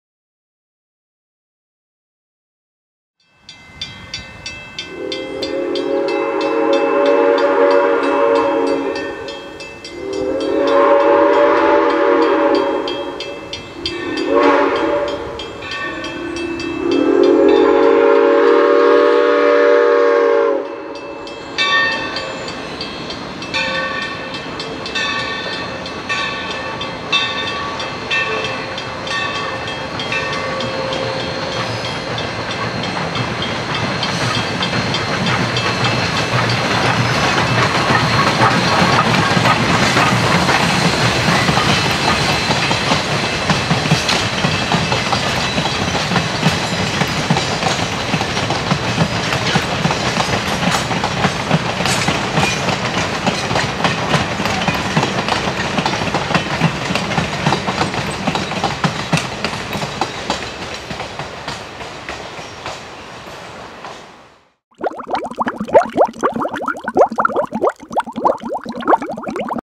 VALLEY RAILROAD Train 8 5 17 2015
Mikado 3025 leads northbound Train 8 at Old Deep River Road in Essex, CT